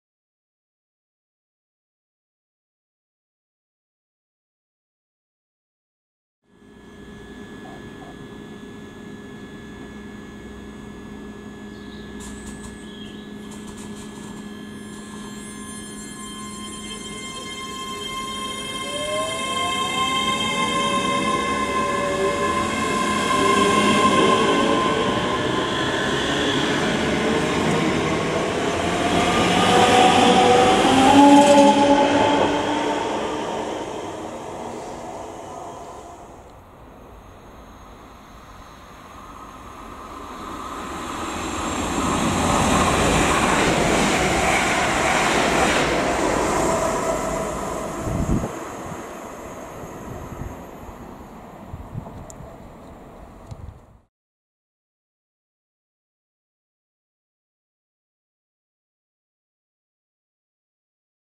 ScotRail Class 385's heading to Edinburgh at Polmont Station: 16/02/23
ScotRail Class 385's at Polmont Railway Station on the 16th of February 2023.